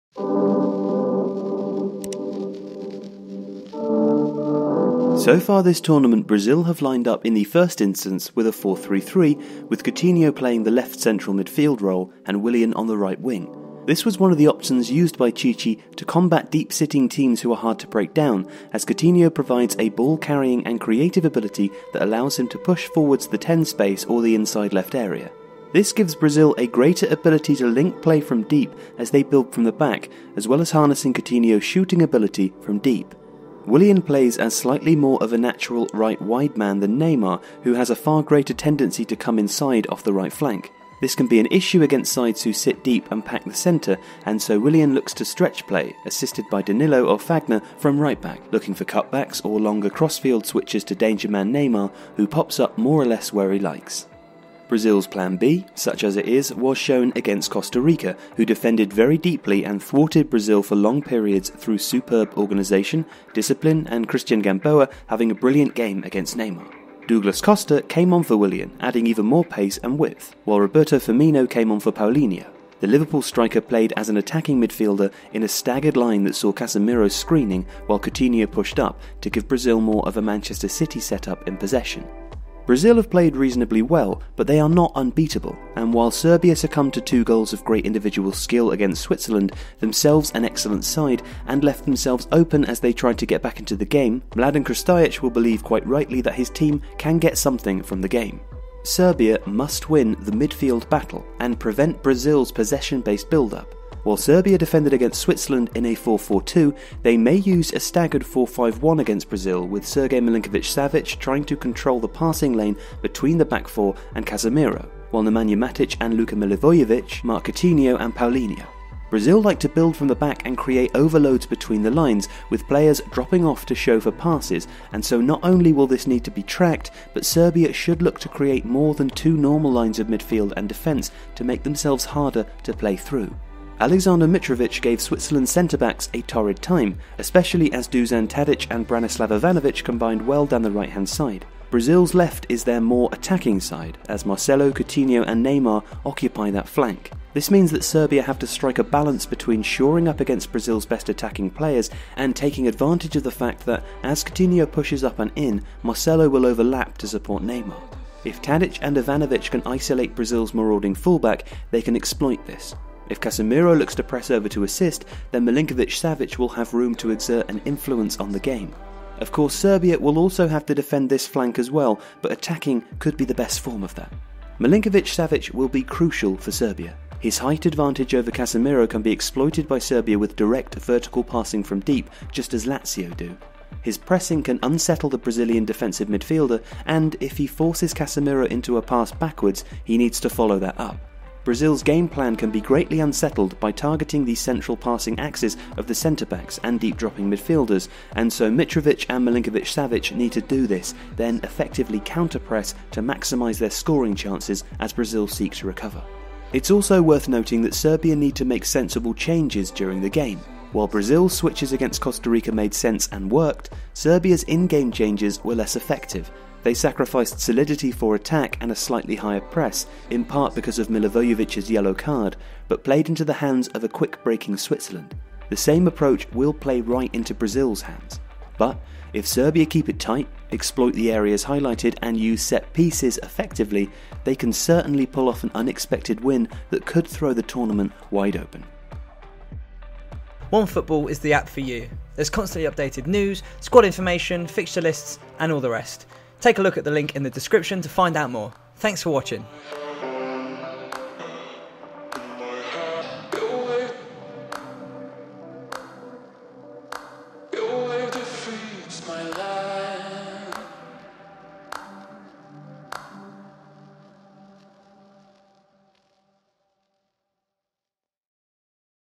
FIFA World Cup 2018™: How Serbia Could Beat Brazil
Serbia face Brazil in their final group stage game with a last 16 berth possible for both. Here, we break down how Serbia could well topple the mighty Brazil and secure their spot in the last 16.

and follow Tifo Football elsewhere

Produced by Tifo Studios: Taking an illustrated look into the beautiful game. Official Media Partner of the Bundesliga.